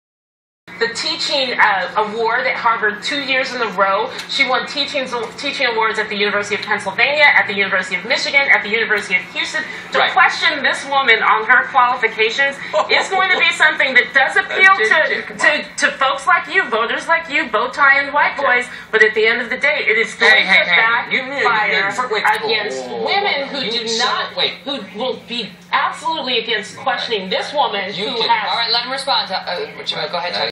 "Bow-tie wearin' White boys?"
Now if Tucker Carlson would have stereotyped Jehmu Greene as the big Black whale that she is, his career would be DONE...her career will now be affirmative-action advanced...such are the ways of vicious, fascistic Leftists